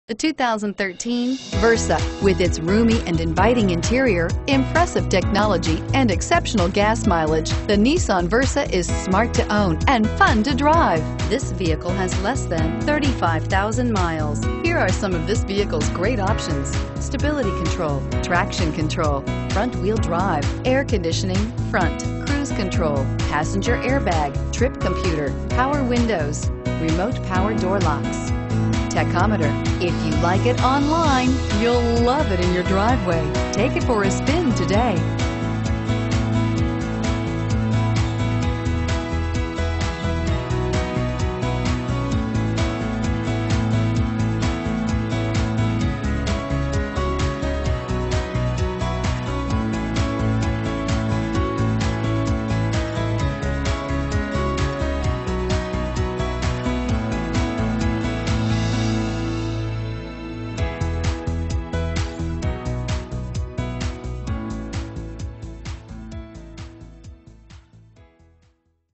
2013 Nissan Versa Chico CA U7747
2013 Nissan Versa 1.6 SV

*This 2013 Nissan Versa SV* will sell fast Oil Changed, and Multi-Point Inspected *Save money at the pump knowing this Versa gets 40.0 MPG!* *This Versa has a very popular Brilliant Silver exterior color with a Charcoal W/Cloth Seat Trim interior that looks fantastic!* *Please let us help you with Finding the ideal New, Used, or Certified vehicle.